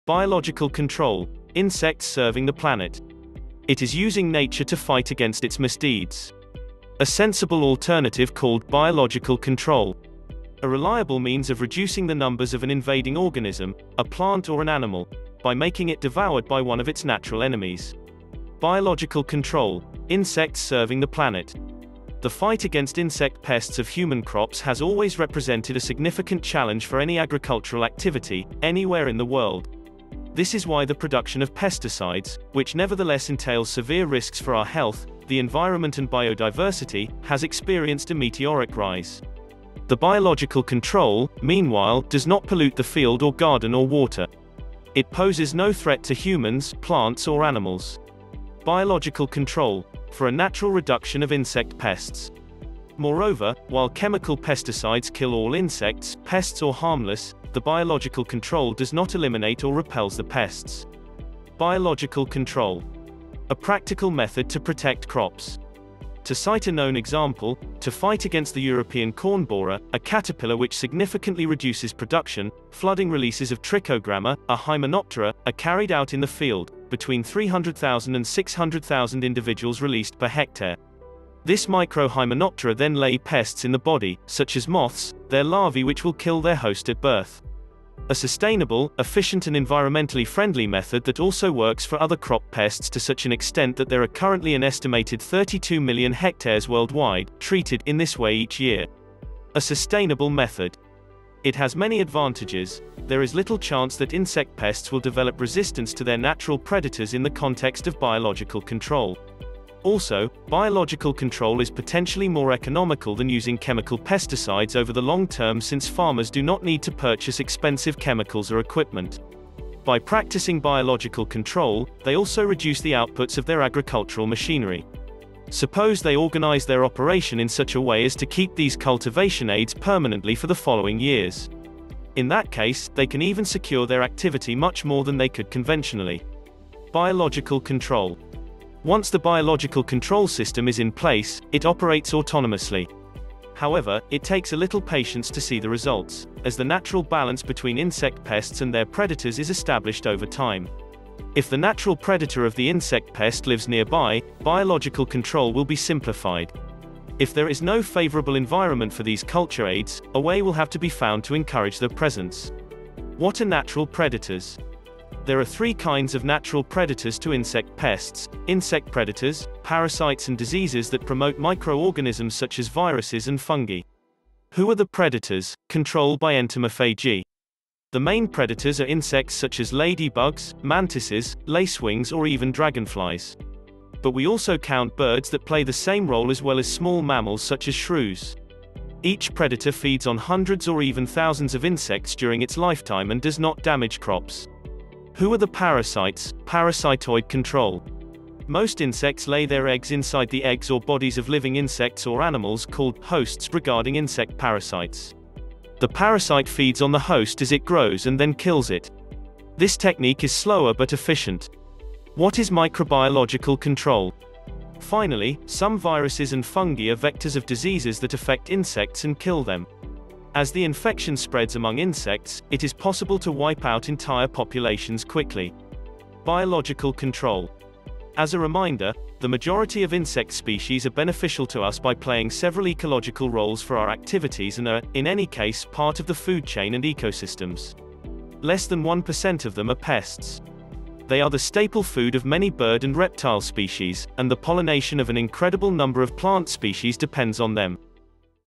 Biological Control Insects Serving The Planet
It is using nature to fight against its misdeeds. A sensible alternative called 'biological control'. A reliable means of reducing the numbers of an invading organism (a plant or an animal) by making it devoured by one of its natural enemies.

The fight against insect pests of human crops has always represented a significant challenge for any agricultural activity, anywhere in the world. This is why the production of pesticides, which nevertheless entails severe risks for our health, the environment and biodiversity, has experienced a meteoric rise. The biological control, meanwhile, does not pollute the field or garden or water. It poses no threat to humans, plants or animals.
Biological control: for a natural reduction of insect pests

Moreover, while chemical pesticides kill all insects, pests or harmless, the biological control does not eliminate or repels the pests.
biological control

A practical method to protect crops

To cite a known example: to fight against the European corn borer, a caterpillar which significantly reduces production, flooding releases of Trichogramma, a Hymenoptera, are carried out in the field (between 300,000 and 600,000 individuals released per hectare). This micro-Hymenoptera then lay pests in the body, such as moths, their larvae which will kill their host at birth.

A sustainable, efficient and environmentally friendly method that also works for other crop pests to such an extent that there are currently an estimated 32 million hectares worldwide " treated " in this way each year.
A sustainable method

It has many advantages: there is little chance that insect pests will develop resistance to their natural predators in the context of biological control. Also, birth control is potentially more economical than using chemical pesticides over the long term since farmers do not need to purchase expensive chemicals or equipment. By practising biological control, they also reduce the outputs of their agricultural machinery. Suppose they organise their operation in such a way as to keep these cultivation aids permanently for the following years. In that case, they can even secure their activity much more than they could conventionally.
biological control

Once the biological control system is in place, it operates autonomously. However, it takes a little patience to see the results, as the natural balance between insect pests and their predators is established over time.

If the natural predator of the insect pest lives nearby, biological control will be simplified. If there is no favourable environment for these culture aids, away will have to be found to encourage their presence!
What are natural predators?

There are three kinds of natural predators to insect pests: insect predators, parasites and diseases that promote microorganisms such as viruses and fungi.
Who are the predators (control by entomophagy)?

The main predators are insects such as ladybugs, mantises, lacewings or even dragonflies.

But we also count birds that play the same role as well as small mammals such as shrews! Each predator feeds on hundreds or even thousands of insects during its lifetime and does not damage crops.
Who are the parasites (parasitoid control)?

Most insects lay their eggs inside the eggs or bodies of living insects or animals called " hosts "regarding insect parasites. The parasite feeds on the host as it grows and then kills it. This technique is slower but efficient.
What is microbiological control?

Finally, some viruses and fungi are vectors of diseases that affect insects and kill them. As the infection spreads among insects, it is possible to wipe out entire populations quickly.
biological control

As a reminder: the majority of insect species are beneficial to us by playing several ecological roles for our activities and are, in any case, part of the food chain and ecosystems. Less than 1% of them are pests.

They are the staple food of many bird and reptile species, and the pollination of an incredible number of plant species depends on them.